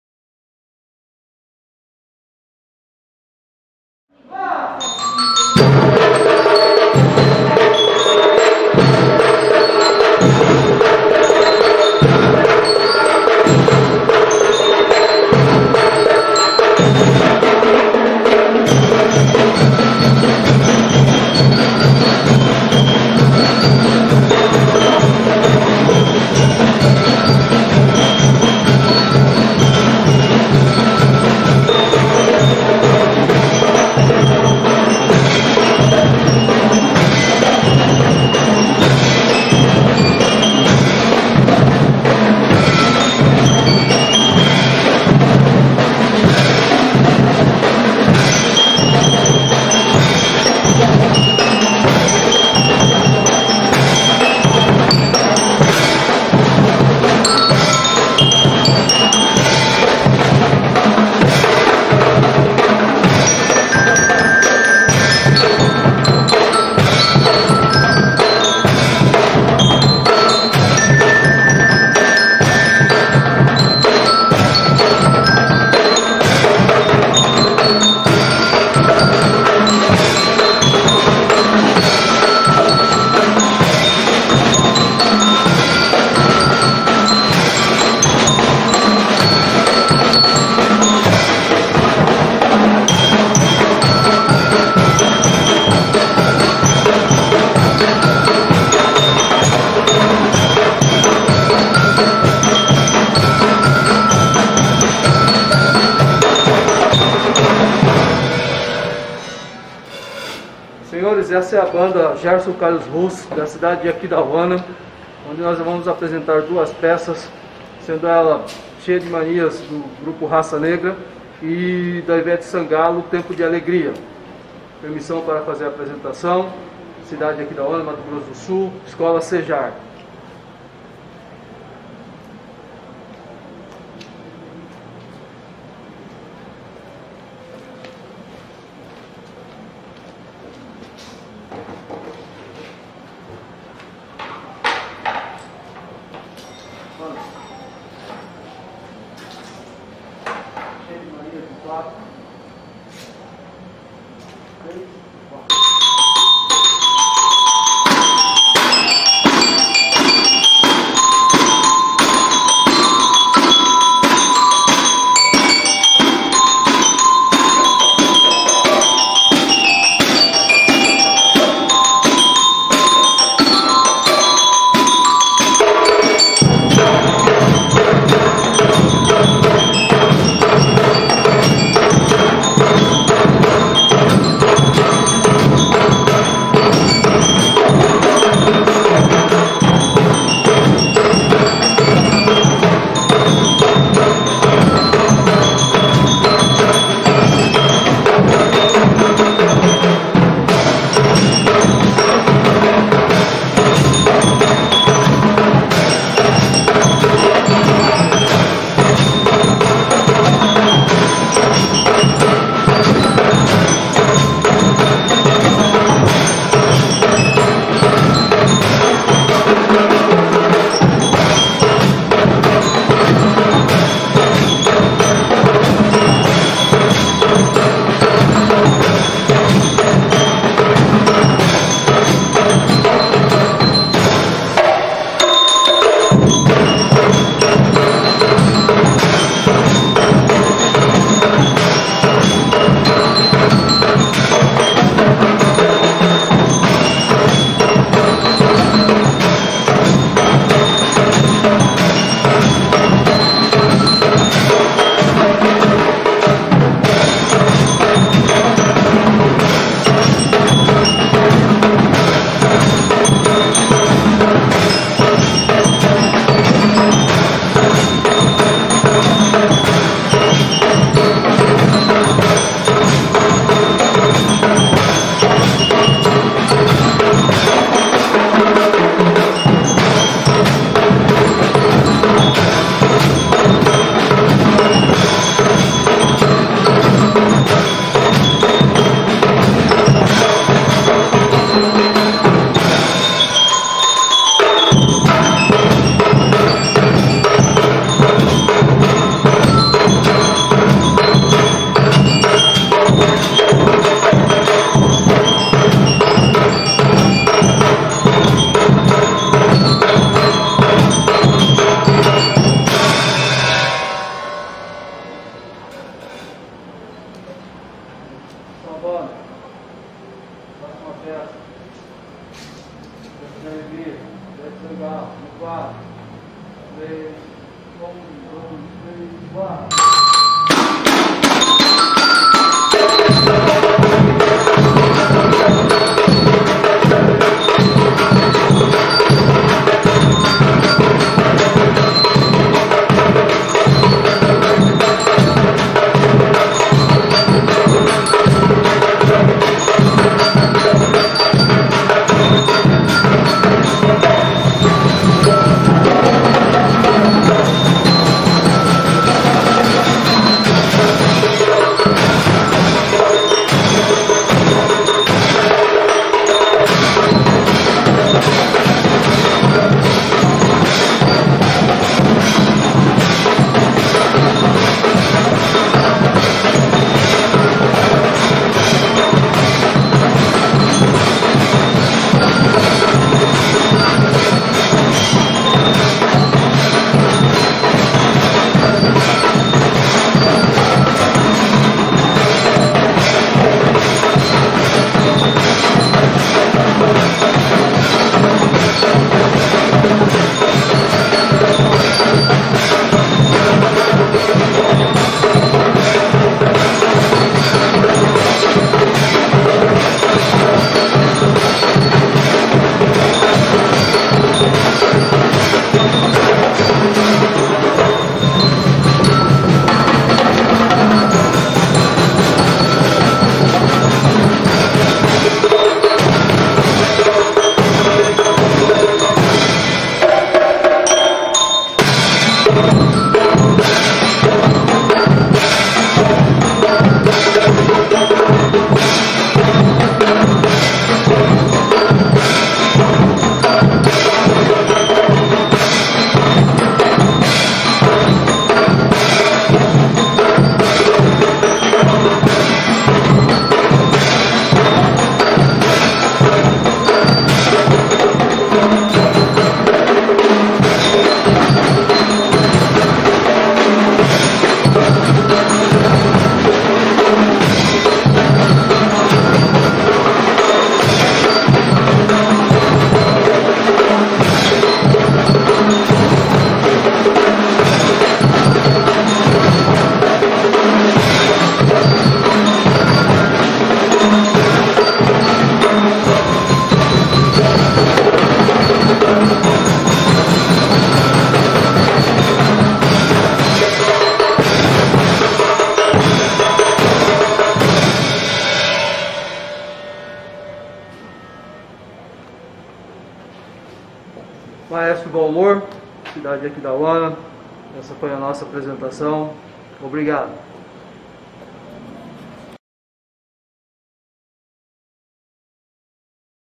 CORPO MUSICAL Banda de Percussão Prof. Gerson Carlos Russi
2° CONCURSO BRASILEIRO VIRTUAL DE BANDAS E FANFARRAS
ABANFAESC 2021
CORPO MUSICAL
Banda de Percussão Prof. Gerson Carlos Russi
Aquidauana/MS
CATEGORIA Banda de Percussão com Liras SÊNIOR